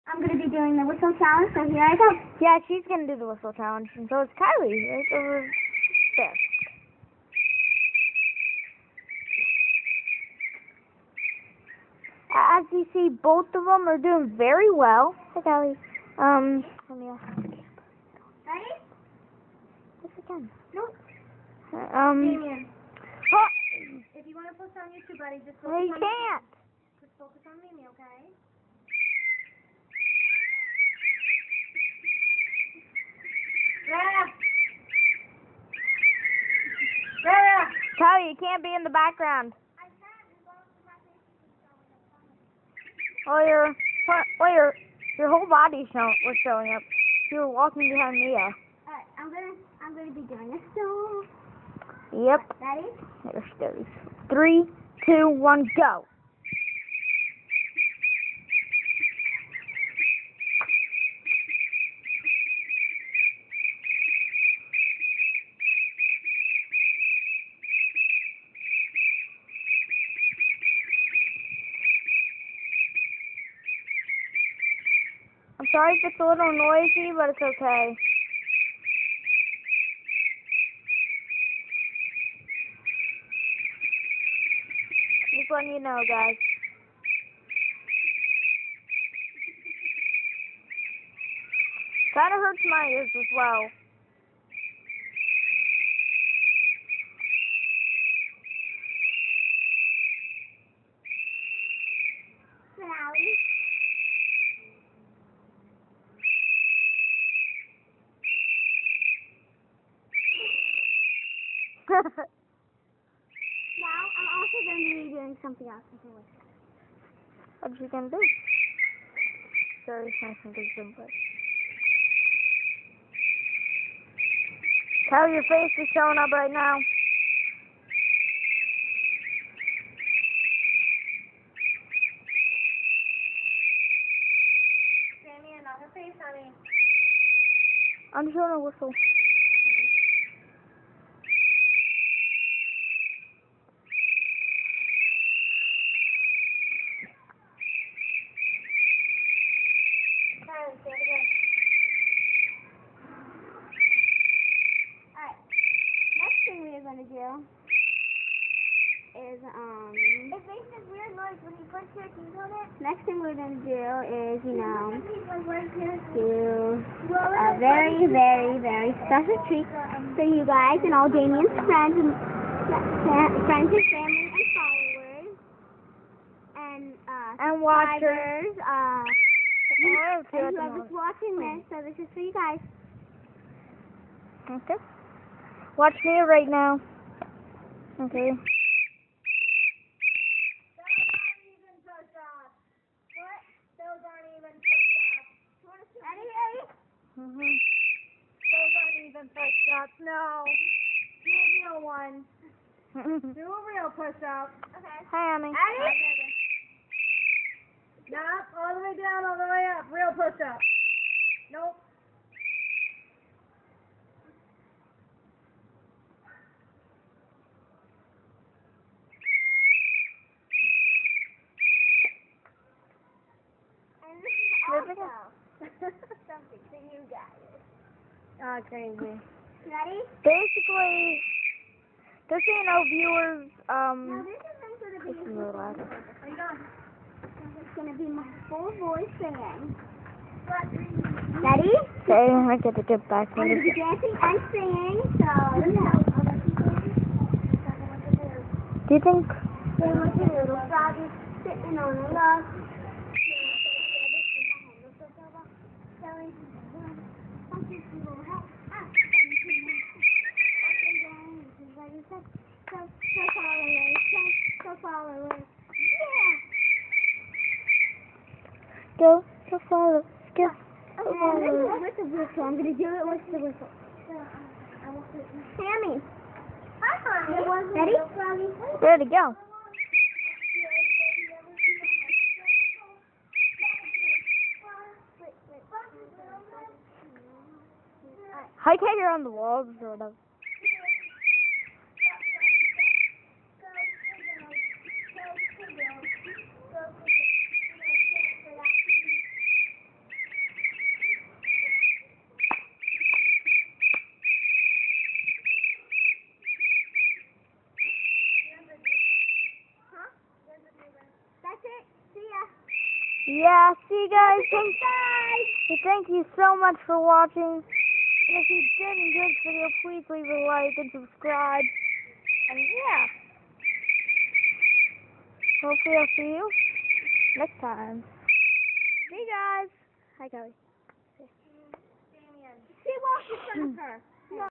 Whistling Songs (17)
Mia and Kylee are whistling  and we also have a new guest. Enjoy.             Let's hit 350 likes!    1000 on my channel.